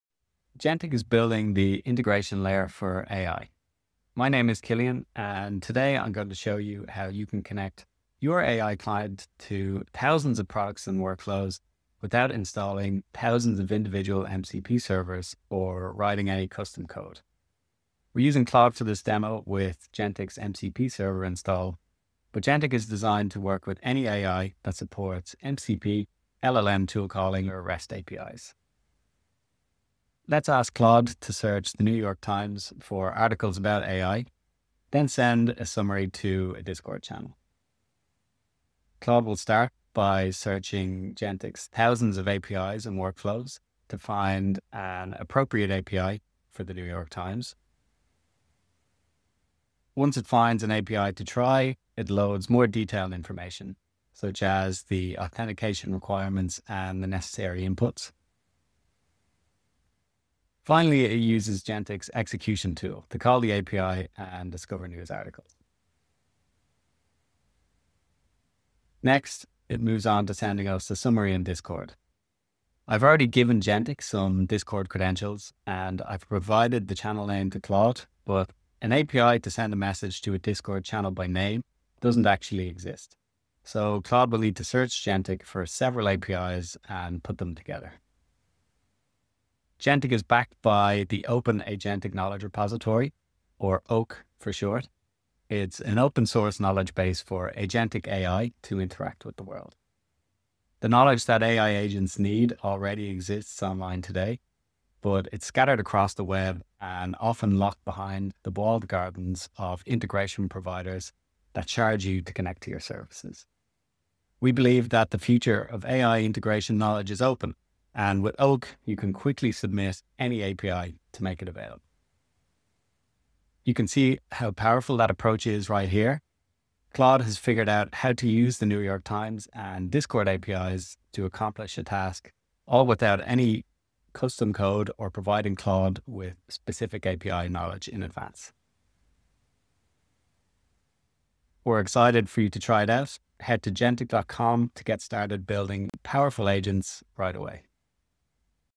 Claude Uses Real APIs with Zero Code — Meet Jentic
In this demo, we show what happens when AI agents can access the real world — no glue code, no plugins, no custom functions.
Using Jentic, Claude connects to the New York Times API, retrieves articles about AI, then posts a summary to Discord — all autonomously, without writing a single line of integration code.

✅ Find and understand APIs
✅ Handle authentication and inputs
✅ Chain multiple APIs together
✅ Execute workflows, no manual setup

This is powered by OAK — the Open Agentic Knowledge repository — and Jentic’s agent-native execution layer.

Try it now and build agents that can actually do things.
👉 Jentic.com